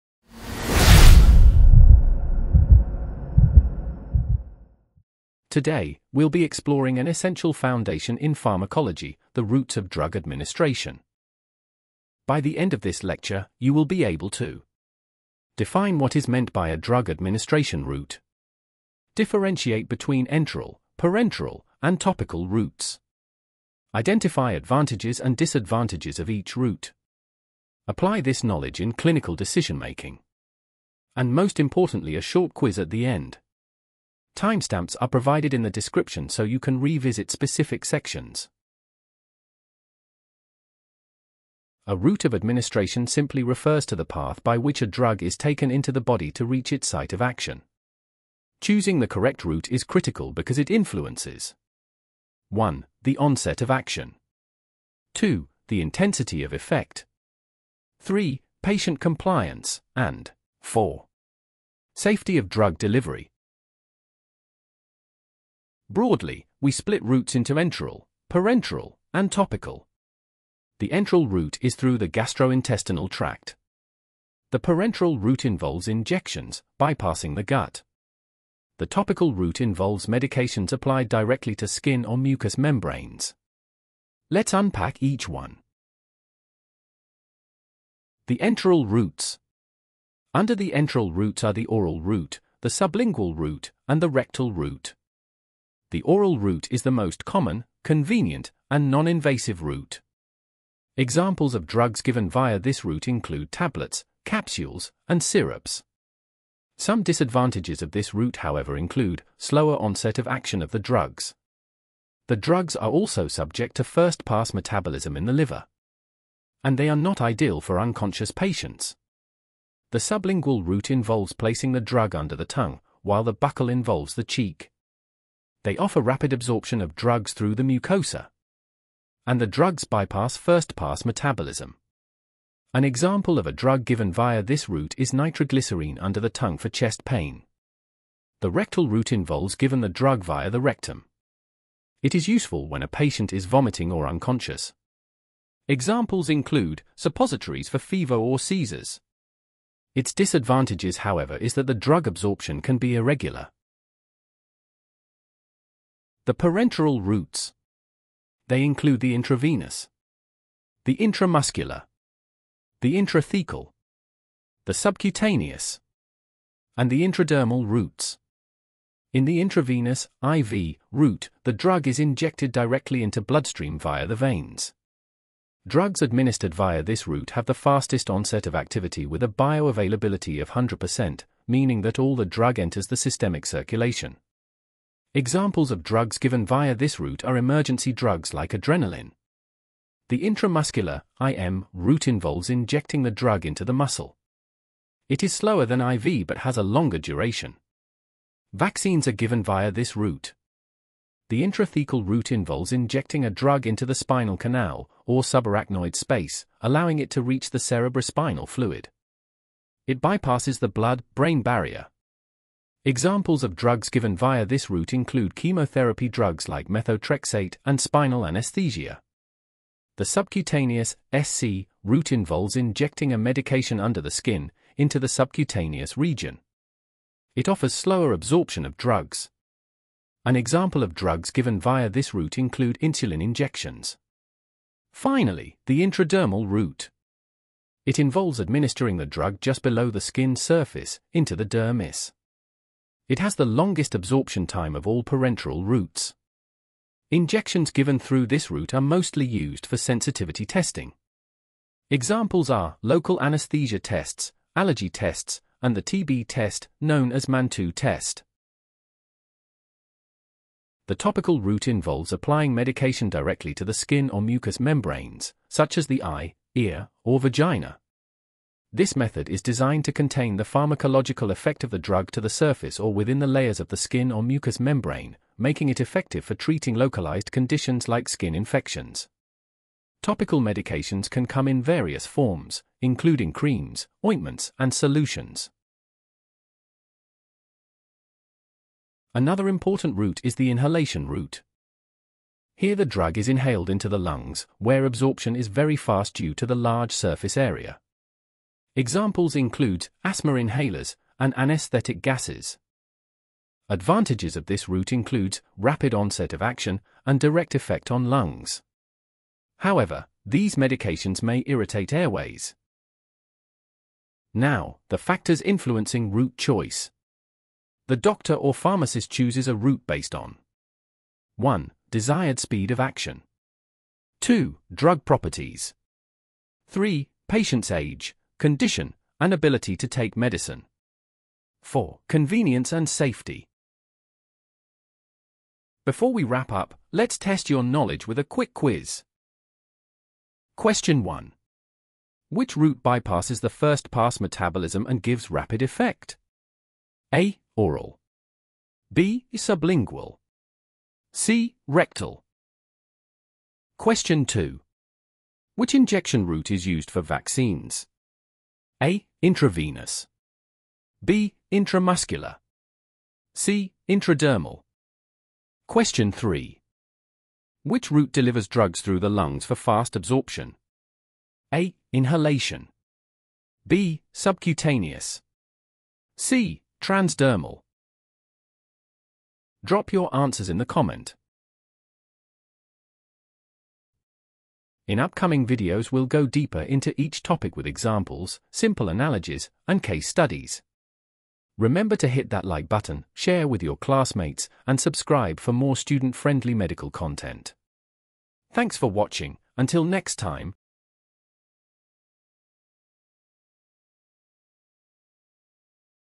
ROUTES OF DRUG ADMINISTRATION / BEST Way to Learn Pharmacology FAST!!
Ever wondered why some medicines come as pills, others as injections, and some as creams, drops, or patches? 💊💉🌿

In this video, we break down the routes of drug administration — the different ways medicines enter the body to work effectively. You’ll learn about:
✅ Enteral routes (oral, sublingual, rectal)
✅ Parenteral routes (IV, IM, SC, intradermal)
✅ Topical and transdermal applications
✅ Inhalation and other specialized routes

We’ll also cover the advantages, disadvantages, and real-life examples of each method. Perfect for students, healthcare professionals, or anyone curious about how medicines work.

Time Stamps

00:00 Intro and overview
00:42 Definition of Routes of Drug Administration and its Significance
01:05 Major Classification of Routes of Administration
01:29 Enteral Route
02:36 Parenteral Route
04:20 Topical Route
04:50 Inhalation Route
05:15 Factors Affecting Route Choice
05:36 Quiz
06:22 Closing

🎯 By the end, you’ll understand why doctors and pharmacists choose a specific route for each drug — and how it impacts speed, safety, and effectiveness.

Answers to Questions

1. B
2. B
3. A